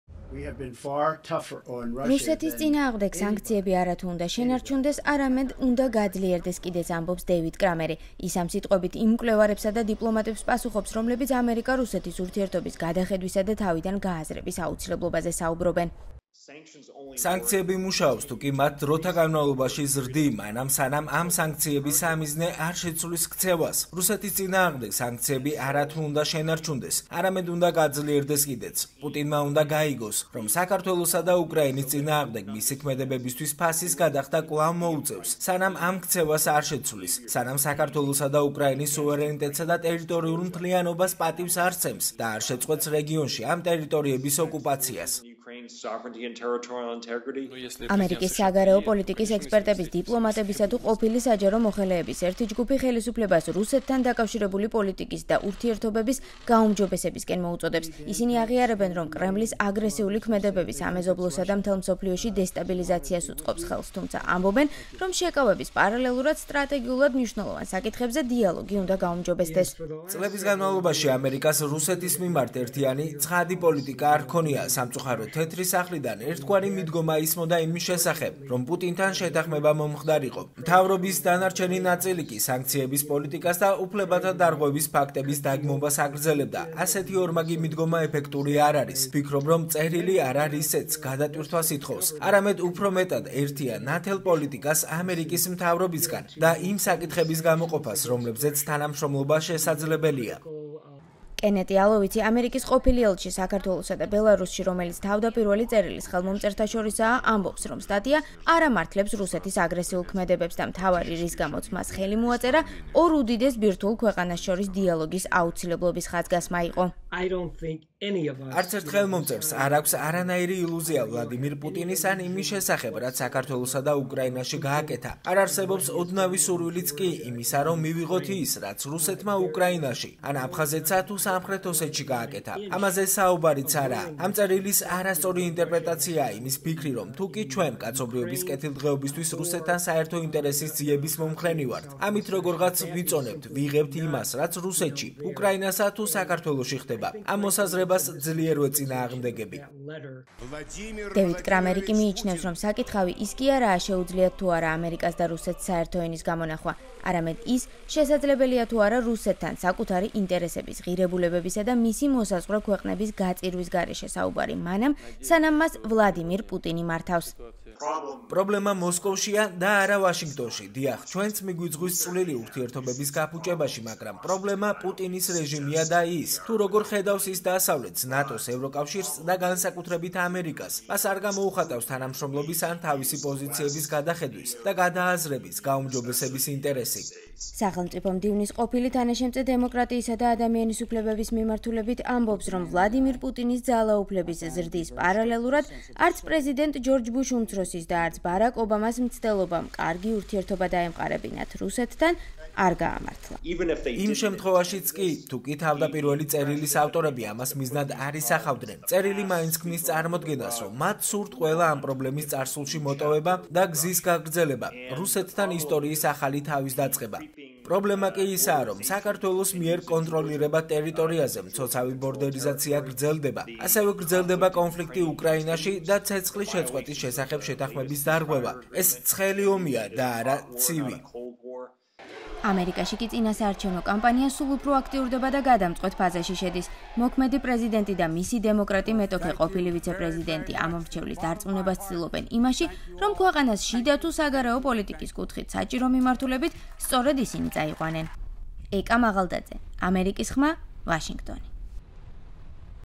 პუტინის ამბიციების გათვალისწინება თუ დიალოგის საჭიროება?
ამერიკელი მკვლევარებისა და დიპლომატების ჯგუფმა ღია წერილი გამოაქვეყნა, რომელშიც ამერიკა-რუსეთის ურთიერთობის გადახედვის აუცილებლობაზე საუბრობენ.მათ მალევე კრიტიკით გამოეხმაურა სხვა ჯგუფი, რომლის წევრებიც მიიჩნევენ, რომ პუტინის რუსეთთან ურთიერთობის გაუმჯობესების მცდელობა კვლავ უშედეგო იქნება.